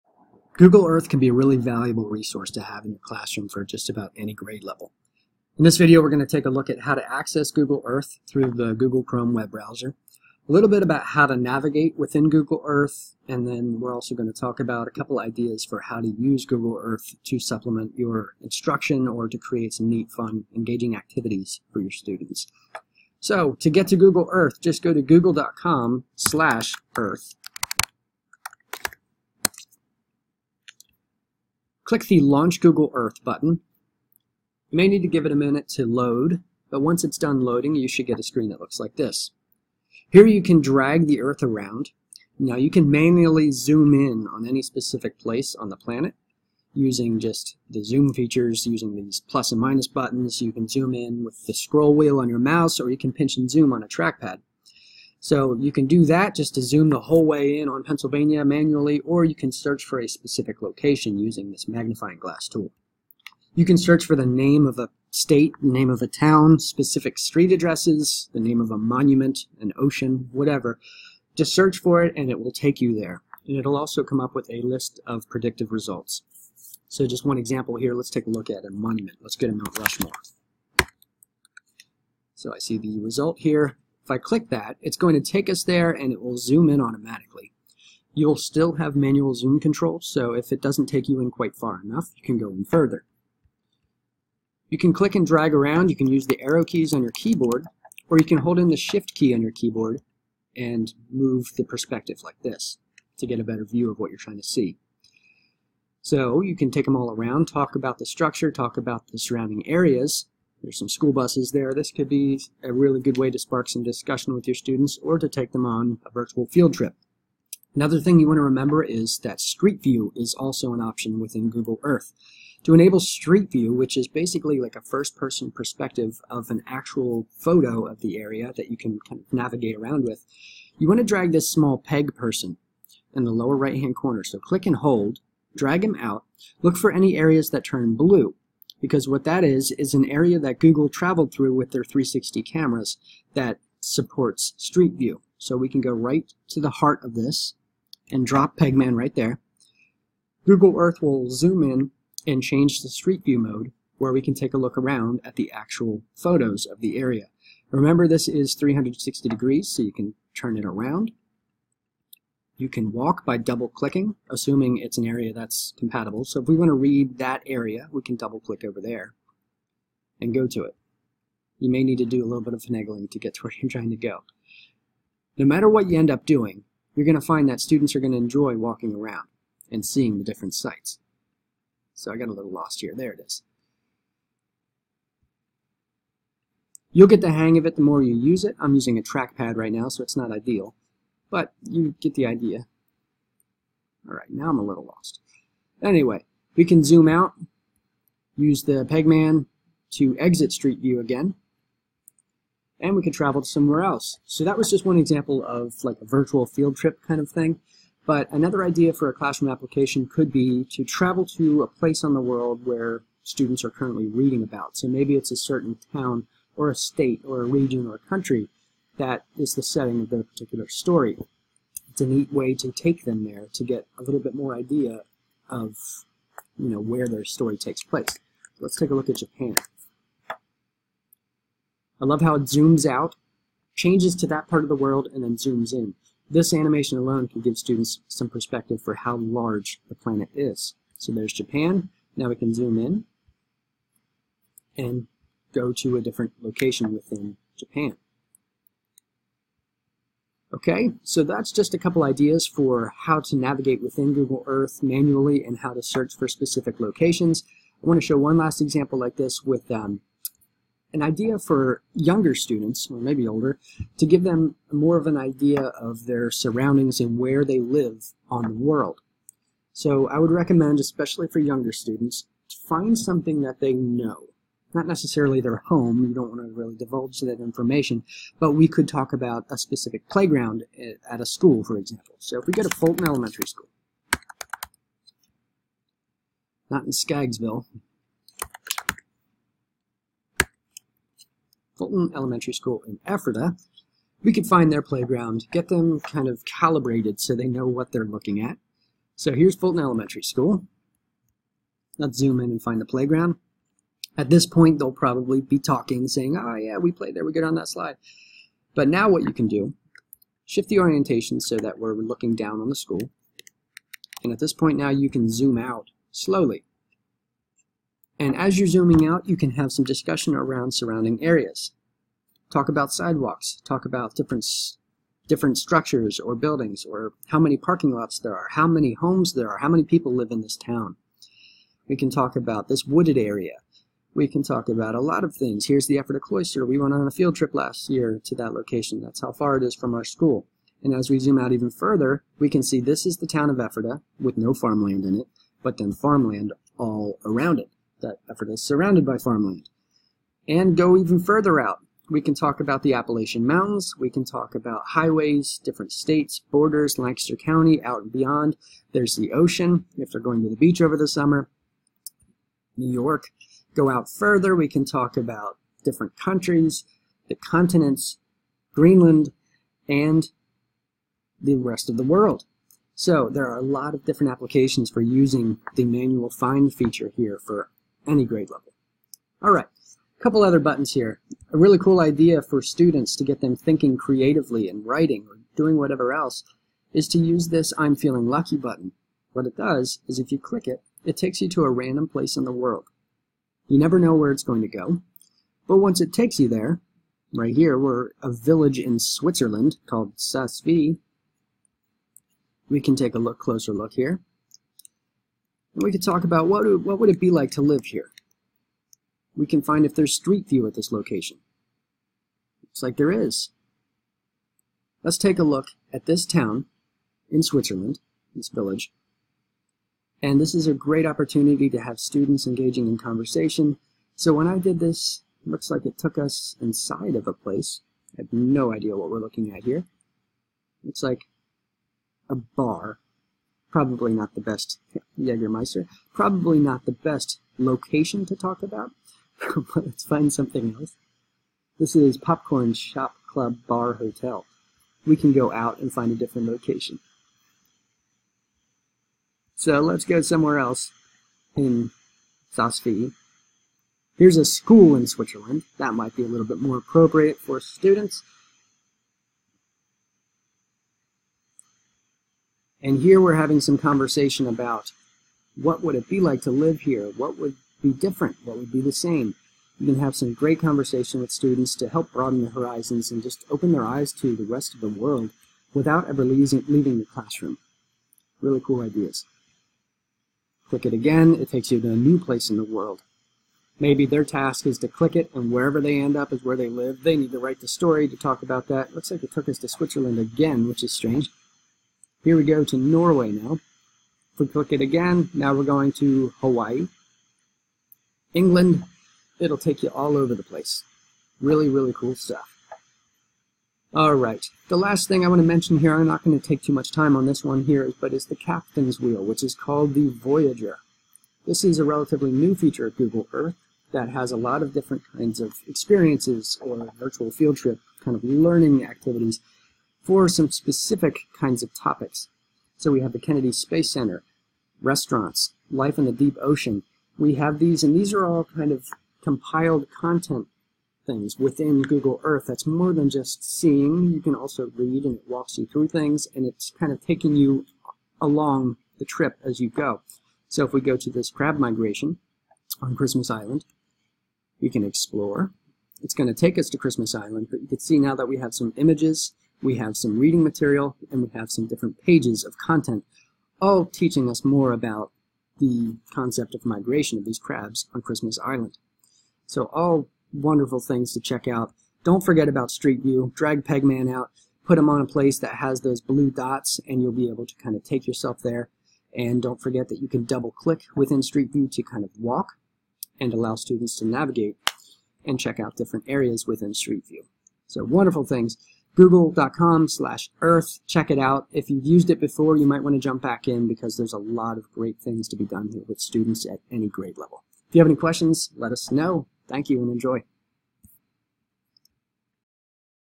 How to Use Google Earth in the Classroom 2018
Learn how to access Google Earth, how to navigate its user interface, and pick up some ideas for how to use it with students!